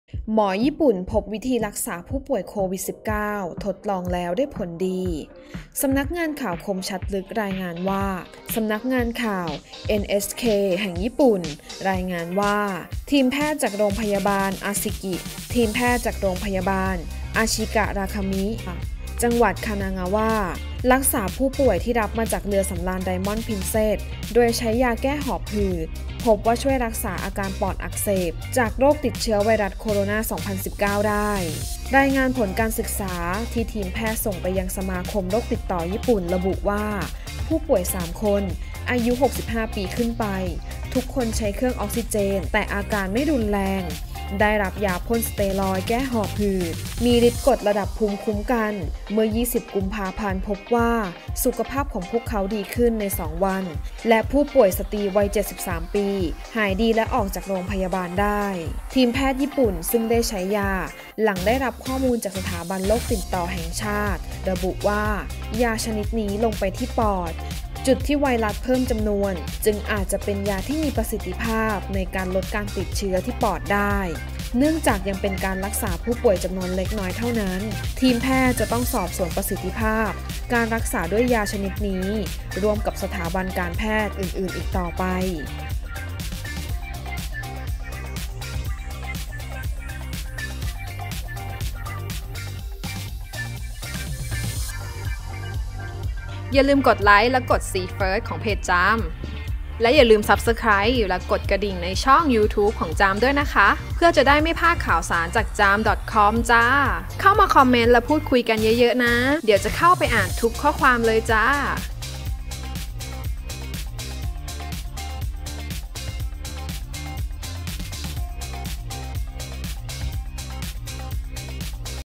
หมอญี่ปุ่น พบวิธี รักษาผู้ป่วยโควิด-19 ทดลองแล้ว ได้ผล
สำนักข่าวคมชัดลึก รายงานว่า  สำนักข่าวเอ็นเอชเค แห่งญี่ปุ่น รายงานว่า ทีมแพทย์จากโรงพยาบาลอาชิกะราคามิ จังหวัดคานางาวะ รักษาผู้ป่วยที่รับมาจากเรือสำราญ ไดมอนด์ พรินเซสส์ โดยใช้ยาแก้หอบหืด พบว่าช่วยรักษาอาการปอดอักเสบจากโรคติดเชื้อไวรัสโคโรน่า 2019 ได้